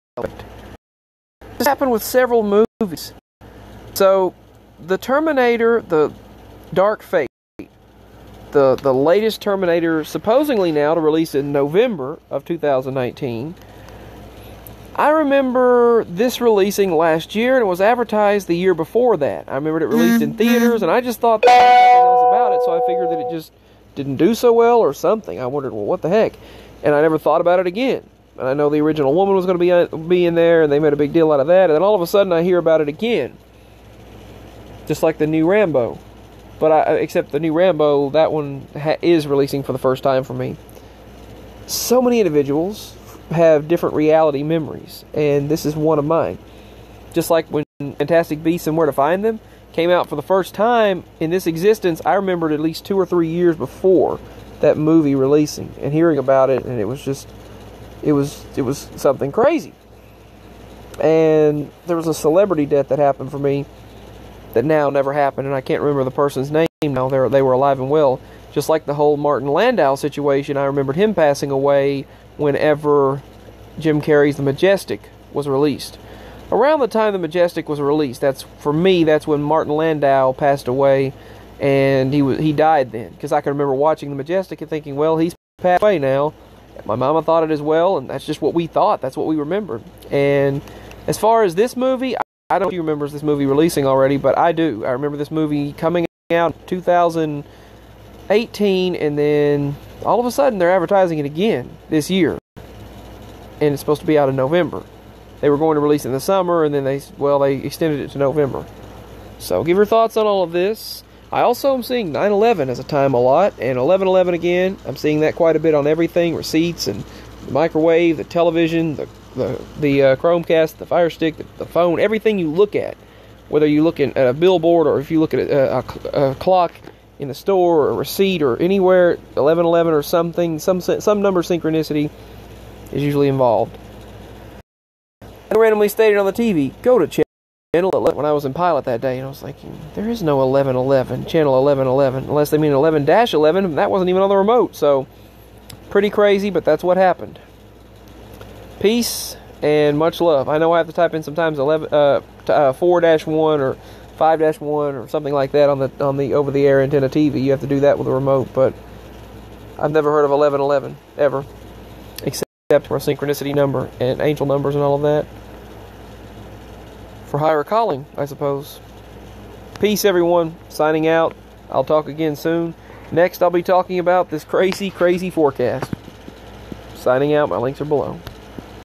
Personal Mandela Effect (New Terminator Released Last Year For Me!)
This is a big personal Mandela Effect for me.  I remember this because the original woman who was in the movie was back again & I remember that last year.  Share your memories!